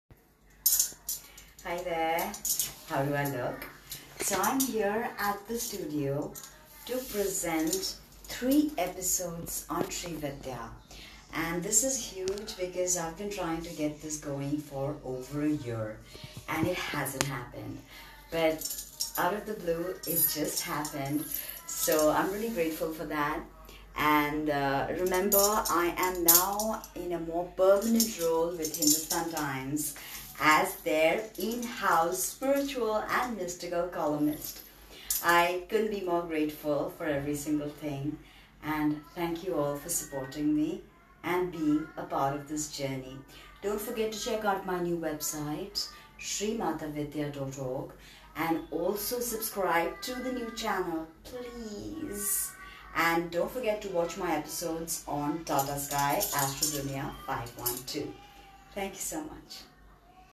At Tata Sky Studio - Before Filming 1 of 3 Episodes on Sri Vidya
Learn more about Sri Vidya, including workshops and deeksha, by following Sri Mata Vidya. You can find all our content through the LinkTree below.